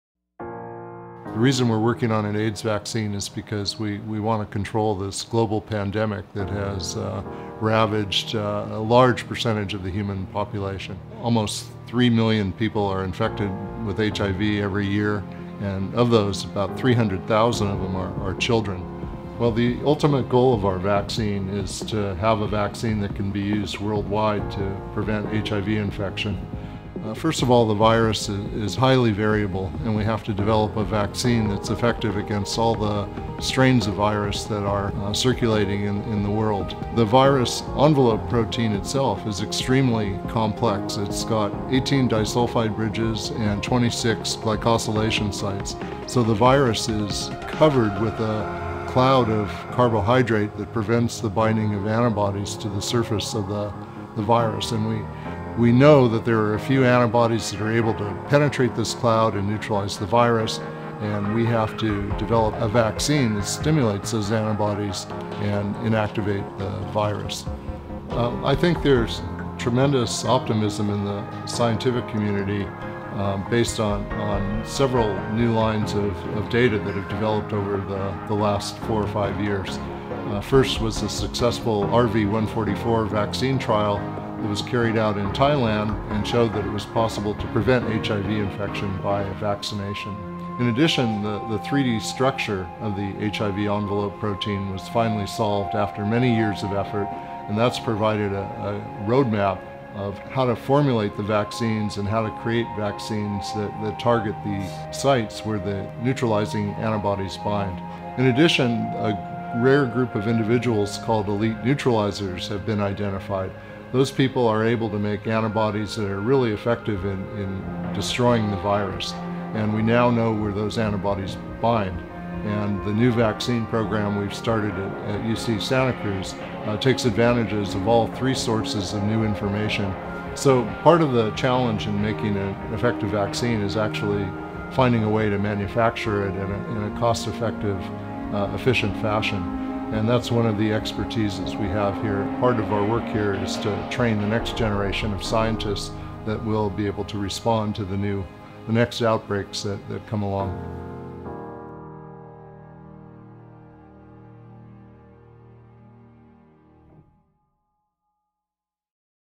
UC Santa Cruz HIV Vaccine Research
Phil Berman, a veteran vaccine researcher now at the University of California, Santa Cruz, explains why he is optimistic about the prospects for an effective AIDS vaccine. Berman's lab has developed new vaccine candidates that he thinks are promising enough to consider advancing into clinical trials within the next two years.